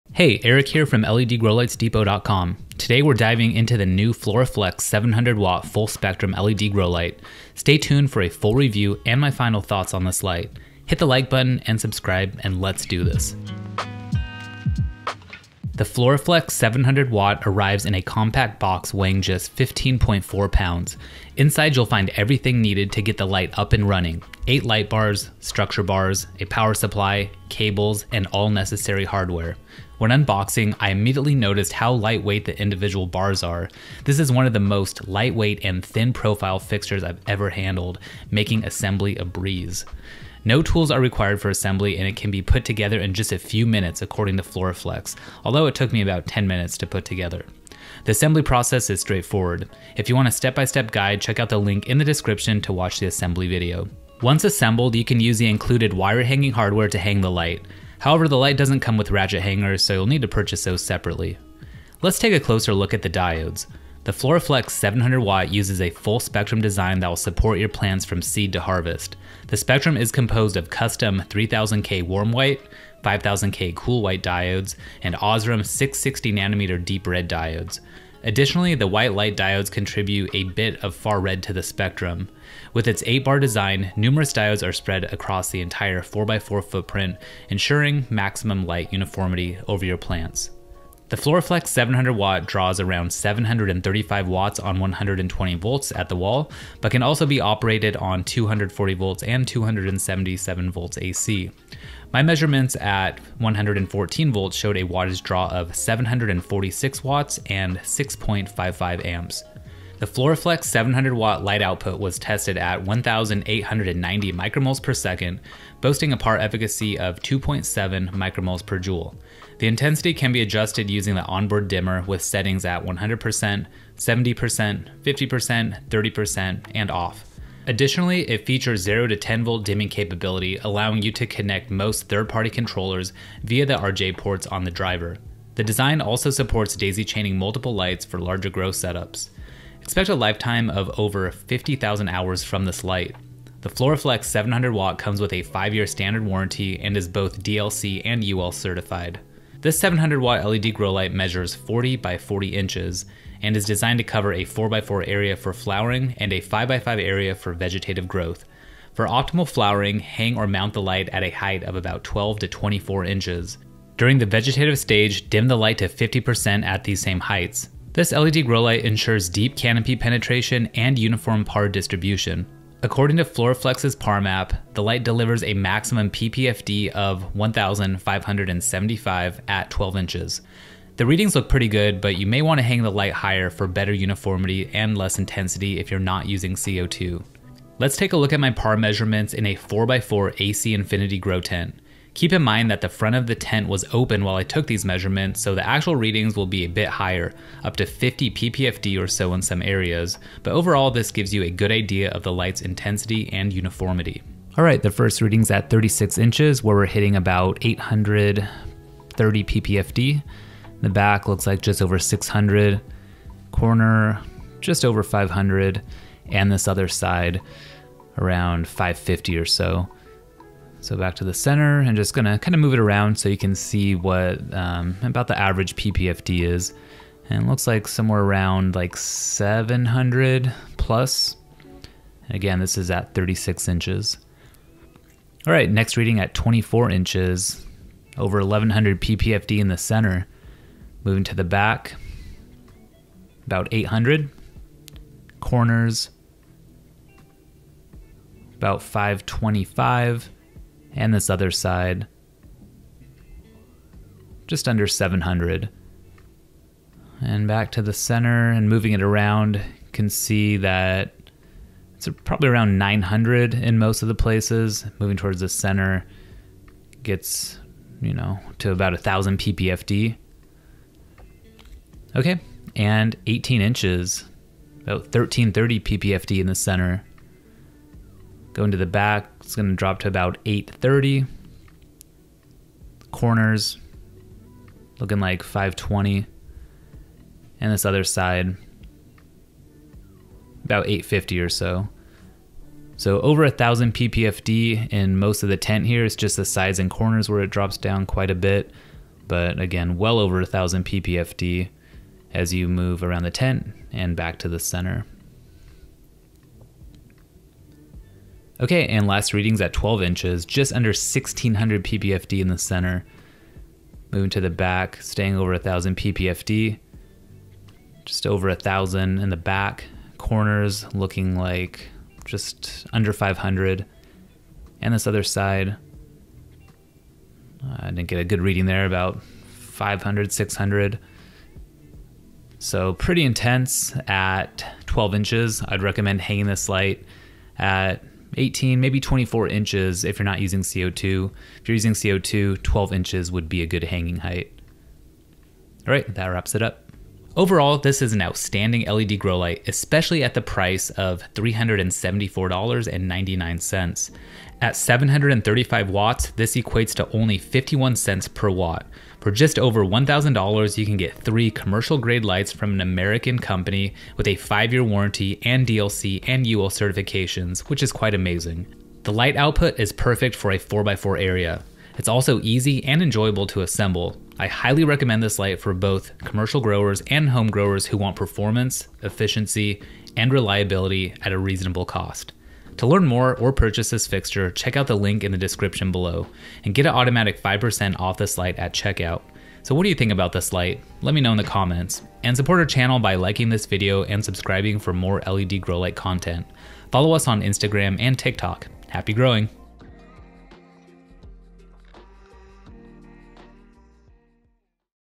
FloraFlex 700W LED Grow Light Review | $0.51/Watt!
I was not paid for this review. All words and opinions are my own.